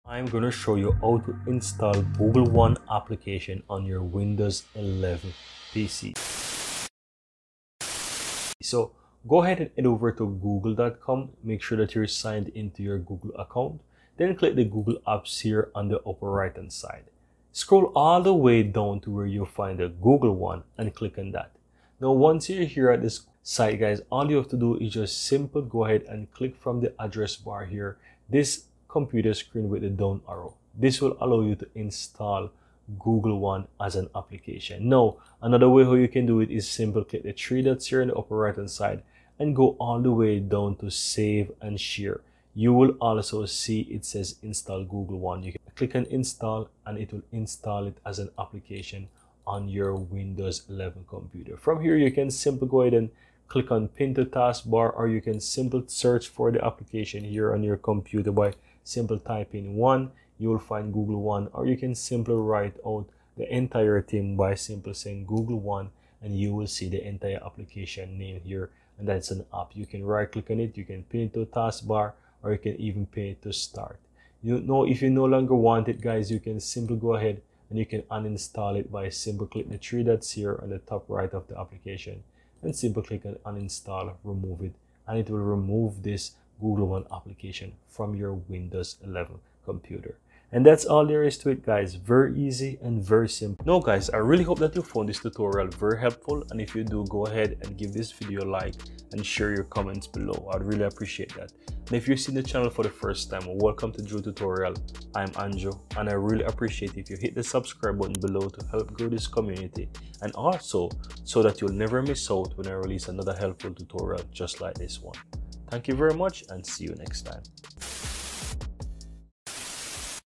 How to Install Google One App on Windows 11
In this tutorial, I show you How to Install Google One App on Windows 11.